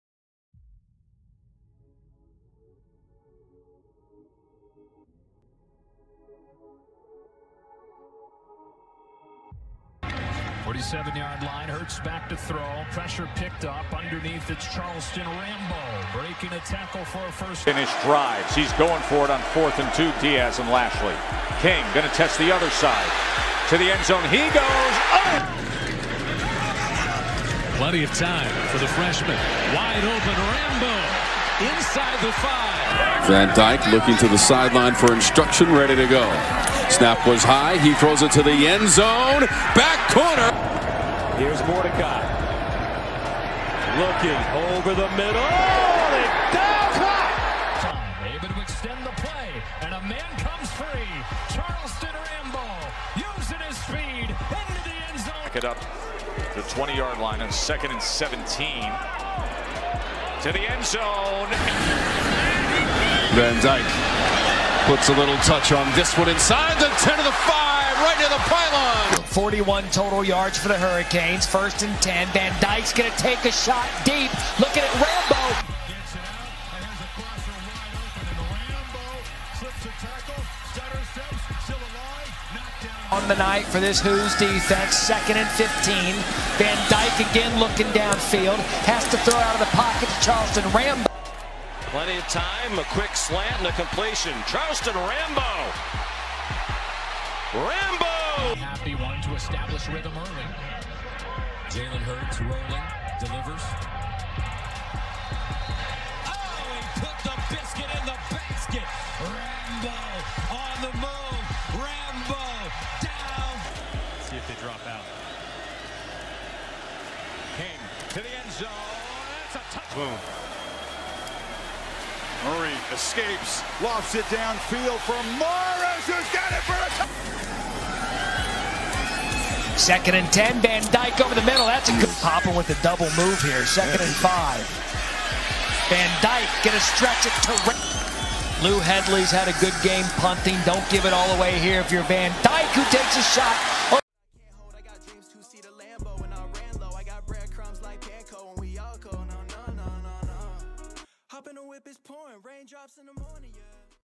MIAMI HURRICANES: Charleston Rambo ᴴᴰ
PLAYER OF THE DAY || MIAMI HURRICANES || CHARLESTON RAMBO HIGHLIGHTS
40 CAREER GAMES
155 RECEPTIONS
2,352 YARDS
16 TOUCHDOWNS